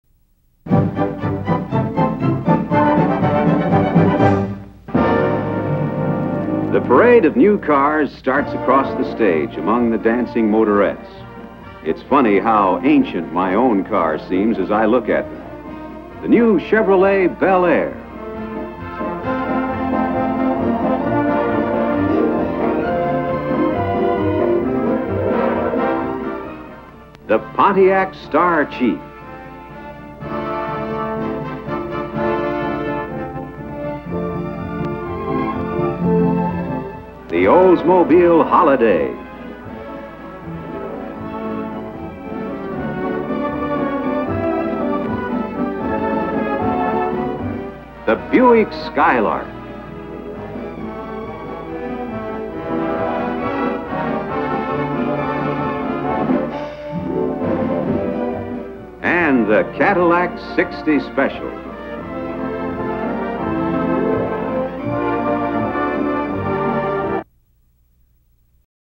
1950s Auto Show with Chevy, Pontiac, Oldsmobile, Buick, and Cadillac
Color footage of a fifties automobile show, showcasing the Chevrolet Belair, Pontiac Star Chief, Oldsmobile Holiday, Buick Skylark, and the Cadillac Sixty Special, 1950s.